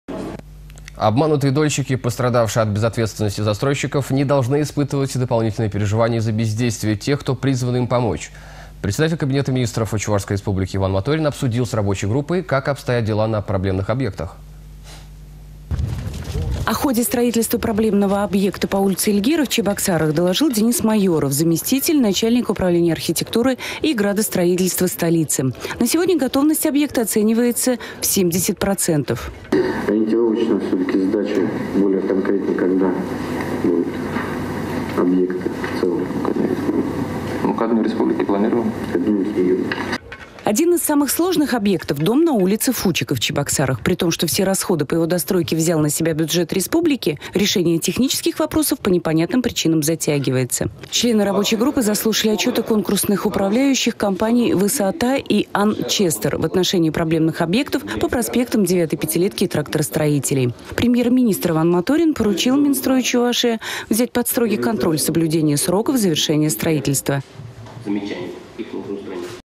Иван Моторин обсудил с рабочей группой сроки завершения строительства проблемных объектов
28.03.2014
Обманутые дольщики, пострадавшие от безответственности застройщиков, не должны испытывать дополнительные переживания из-за бездействия тех, кто призван им помочь.  Председатель Кабинета Министров Чувашской Республики Иван Моторин обсудил с рабочей группой сроки завершения строительства проблемных объектов.  В совещании приняли участие представители сетевых компаний, инициативные группы обманутых дольщиков.